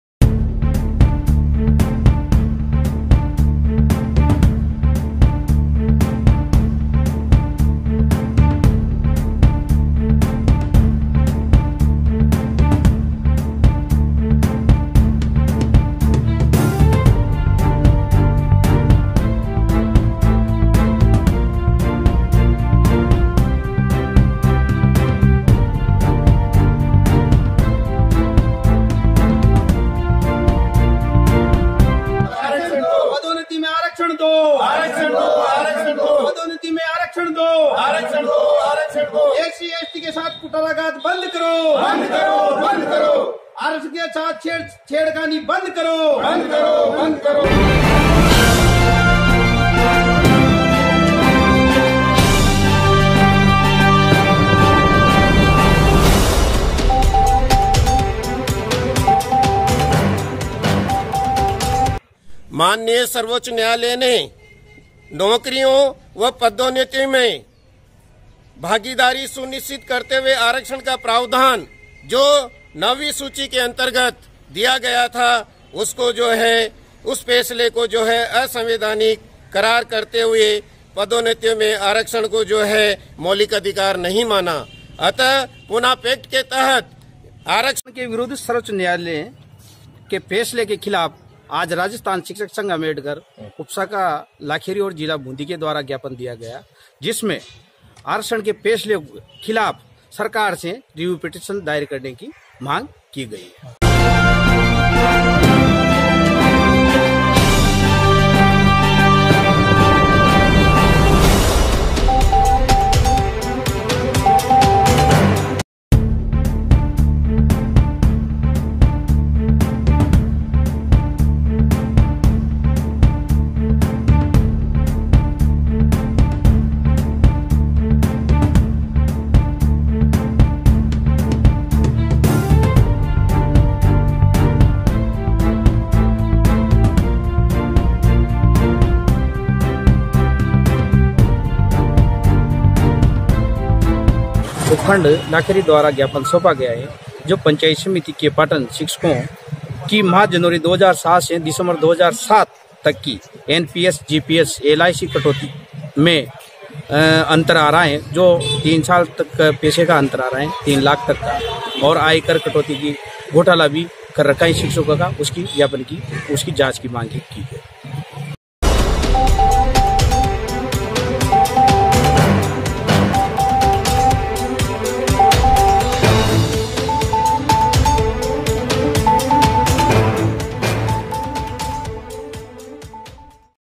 राष्ट्रपति के नाम ज्ञापन सौंपा लाखेरी - सोमवार को राजस्थान शिक्षक संघ (अंबेडकर) बूंदी राजस्थान
राष्ट्रपति के नाम ज्ञापन सौंपा
लाखेरी - सोमवार को राजस्थान शिक्षक संघ (अंबेडकर) बूंदी राजस्थान के जिलाध्यक्ष राजाराम मेघवाल के नेतृत्व में संघ के सदस्यों ने राष्ट्रपति के नाम ज्ञापन सौंपा। ज्ञापन में बताया गया है कि भारत में निर्धन सामान्य वर्ग, पिछड़ों, अति पिछड़ों, अनुसूचित जाति व अन्य जनजातियों की दशा सुधारने व समुचित विकास के लिए सार्वजनिक सेवाओं में नियुक्ति में संविधान के अनुच्छेद 15(4), 16(4), 16(4 क) व 16(4 ख) के तहत नौकरियों व पदोन्नतियो में इनकी भागीदारी सुनिश्चित करते हुए आरक्षण का प्रावधान किया गया था लेकिन हाल ही में उत्तराखंड राज्य के पदोन्नति में आरक्षण के मामले को लेकर माननीय सर्वोच्च न्यायालय ने और असंवैधानिक फैसले में कहा कि सार्वजनिक सेवाओं, नियुक्तियों व पदोन्नति में आरक्षण मौलिक अधिकार नहीं है और राज्य सरकार द्वारा आरक्षण देना राज्य का संवैधानिक कर्तव्य नहीं है। ज्ञापन में सर्वोच्च न्यायालय के उक्त असंवैधानिक फैसले को संघ ने गलत बताते हुए आक्रोश व्यक्त करते हुए बताया कि अनुसूचित जाति व जनजाति वर्ग को पूना पैक्ट के अंतर्गत आरक्षण का प्रावधान पर्याप्त प्रतिनिधित्व व उनकी भागीदारी सुनिश्चित करने के लिए किया गया था। अतः राजस्थान शिक्षक संघ (अंबेडकर) ने ज्ञापन के माध्यम से सरकार को सर्वोच्च न्यायालय में रिव्यू पिटीशन दाखिल करने कि मांग की जिससे और संविधान संशोधन द्वारा उक्त असंवैधानिक फैसले को खारिज करते हुए नौकरियों व पदोन्नति में आरक्षण बहाल करते हुए इसे 9 वीं अनुसूची में सम्मिलित किया जा सके। ज्ञापन में बताया कि यदि सरकार द्वारा उक्त कार्यवाही नहीं की गई तो संघ द्वारा आरक्षण के संरक्षण के लिए समस्त समाज को साथ लेकर सड़कों पर जन आंदोलन करने के लिए मजबूर होगा। इस मौके पर चंद्र मोहन वर्मा अध्यक्ष लाखेरी, अमर सिंह बेरवा अध्यक्ष इंदरगढ़, मुरारी लाल मीणा, महावीर प्रसाद मीणा, राम कुमार बेरवा, बनवारी लाल खटीक संगठन मंत्री, राधेश्याम तमोली, नरेंद्र कुमार सहित कई अध्यापक मौजूद रहे।

प्रयाग राज शर्मा, लाखेरी, जिला बूंदी राजस्थान